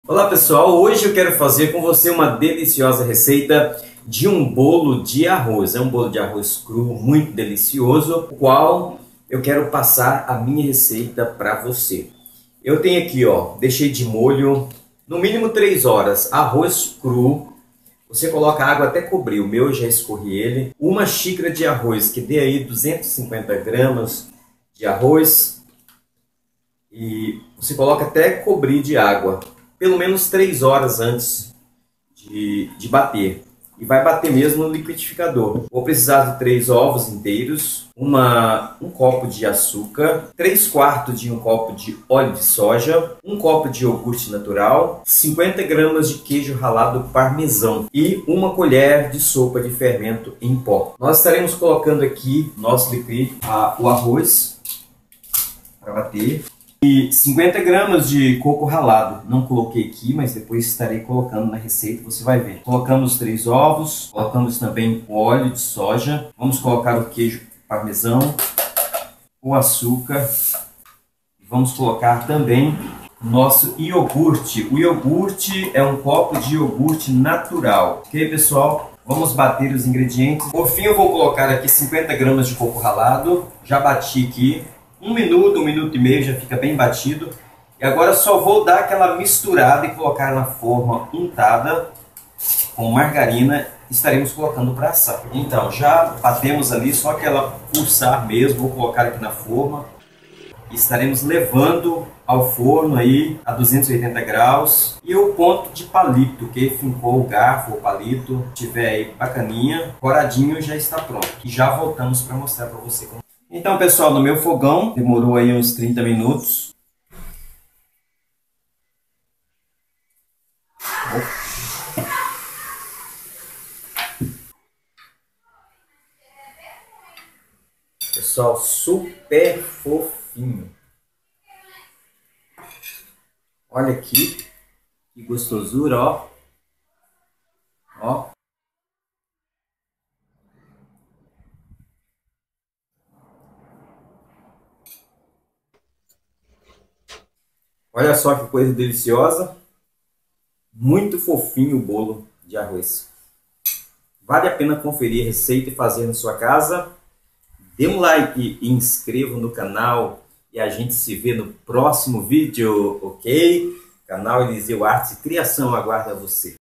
Bolo de Arroz fofinho
um delicioso bolo, super facil de fazer
1 xicara de arroz cru de molho por 3 horas
1 copo americano de açucar
3/4 de um copo de oleo
1 copo de iogurte natural
3 ovos inteiros
50 gramas de queijo ralado parmesão
50 gramas de coco ralado
1 colher de sopa de fermento
Nome: Elizeu Correia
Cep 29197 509
Caixa Postal comunitária Nº31
Posto Correio – Barra do Riacho
Aracruz – ES